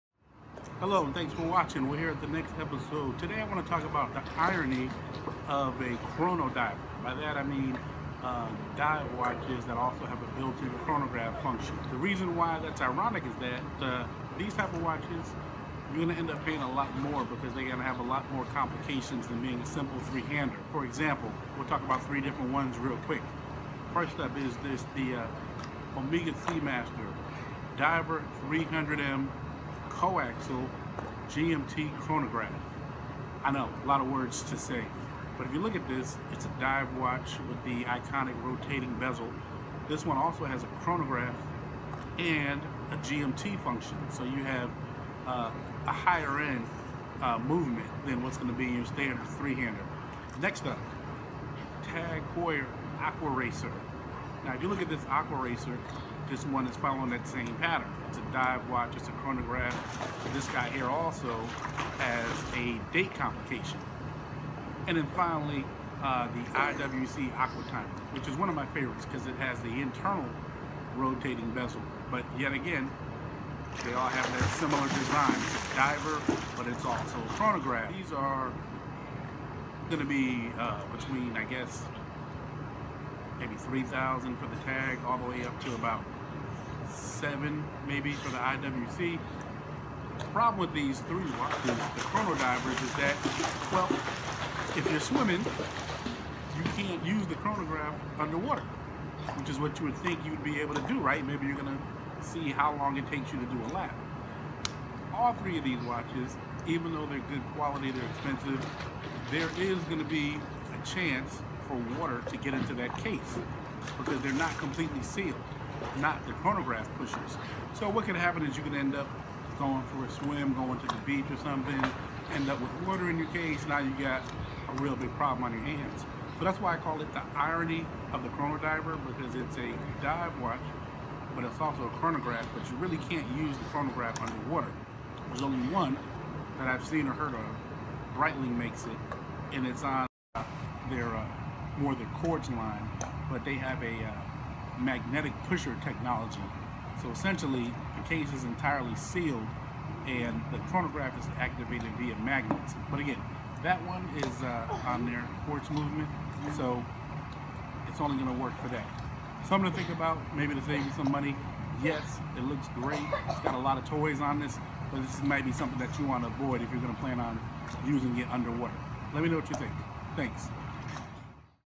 Why Dive Watches with Chronographs are Dumb
Sometimes water and watches watches just aren't a good combination.  This is why you should never use a chronograph underwater.